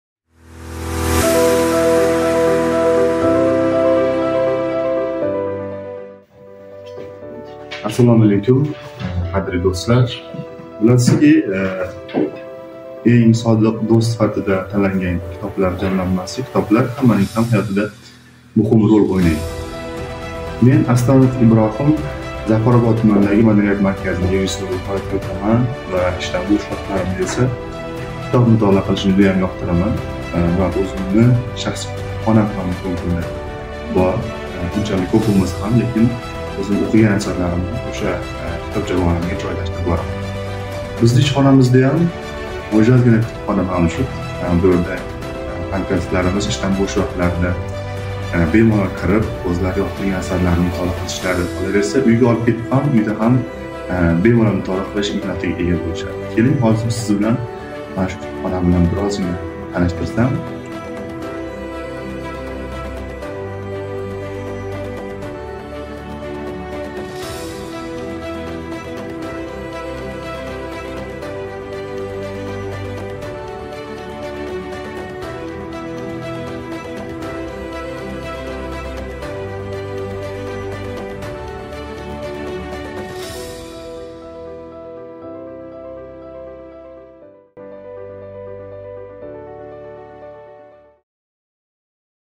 Madaniyat markazi kutubxonasi faoliyatini e'tiboringizga havola etamiz.
📚 Chekka hududlardagi mahalla kutubxonlari.
Zafarobod tumani hududidagi joylashgan Madaniyat markazi kutubxonasi faoliyatini e'tiboringizga havola etamiz.